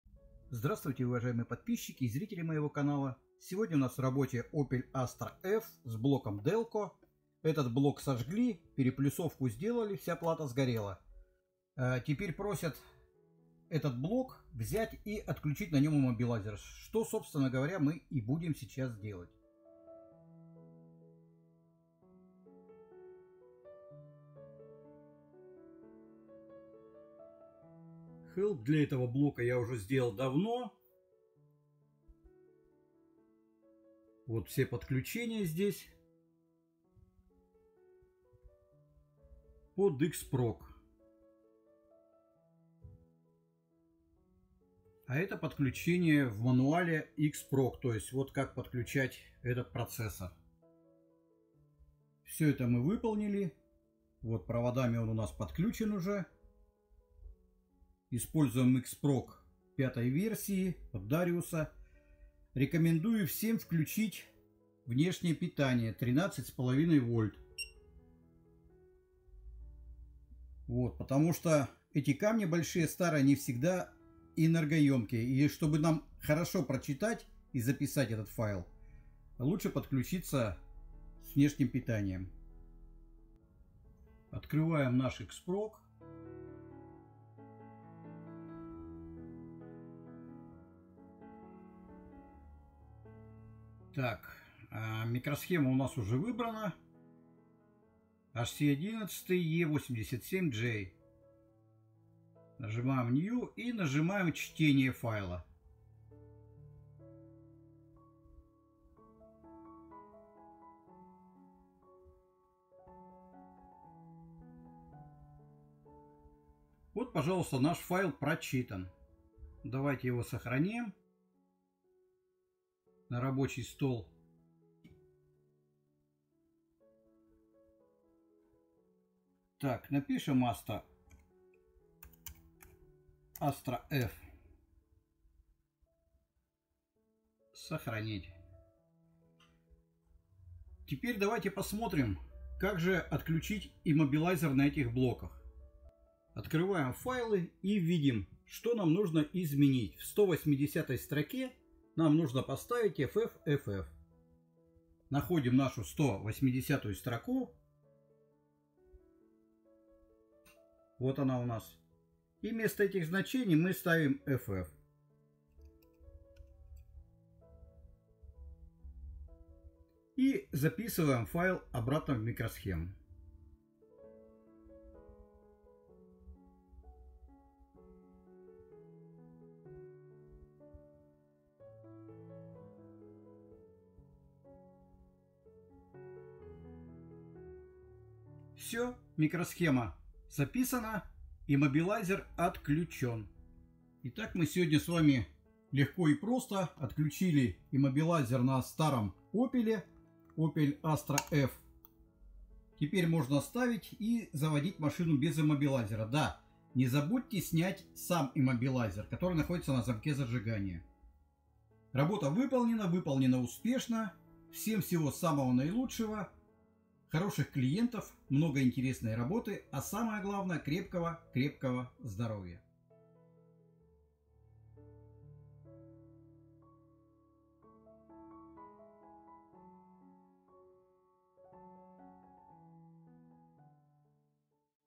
Опель Астра F (Opel Astra F) как отключить иммобилайзер Delco ECU. Отключаем с помощью X-Prog M 5.0
Сожгли ЭБУ DELCO, привезли новый ЭБУ и попросили отключить иммобилайзер! Читаем и записываем процессор HC11 с маской E87J с помощью X-Prog M 5.0
Наши контакты:
Качественный чип-тюнинг, удаление и восстановление различных компонентов!